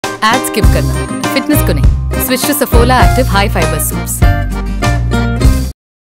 Saffola Active Soups - Stay fit with high fibre
All new Saffola Active Soups is now available in 5 lip-smacking flavours which have 5x more fibre than any other soups! Indulge in the soupy delight and feel full with high fibre.

Follow Saffola Fit Foodie on Twitter: @theFit_Foodie

Category: Entertainment, People & Blogs